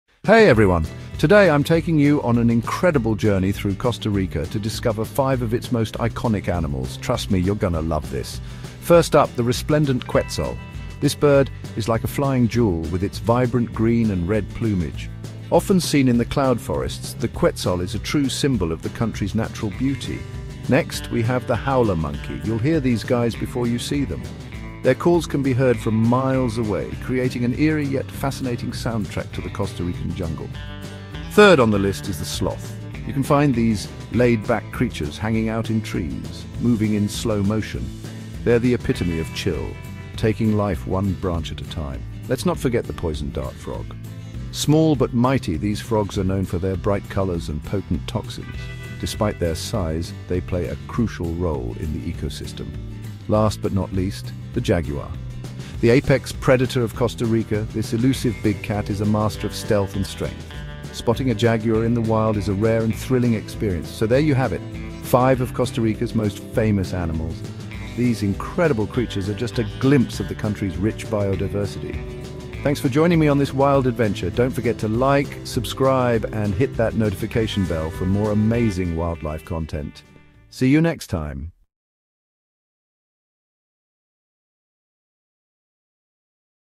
Dr. Agus Sofyan: Five iconic animals in Costa Rica Rainforest
Cloud Forest Costa Rica five iconic animals.
The last small dart-frogs are dangerous.